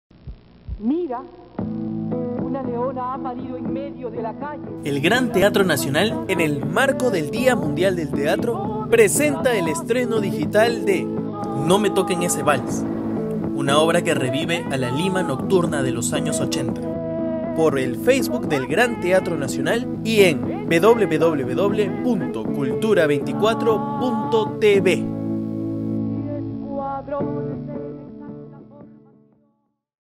No me toquen ese valse de Yuyachkani (PROMO)
[TRANSMISIÓN] ¡Este domingo, por el Día Mundial del Teatro!

Déjate atrapar por esta obra de Los Yuyas, la página de Yuyachkani, en la que dos cantantes muertos, no se sabe cómo, regresan al bar donde actuaban como una aparición a los parroquianos. 😱

📆 Domingo 27 de marzo, 4:30 p.m.
👉 Transmisión en Facebook y Cultura24.tv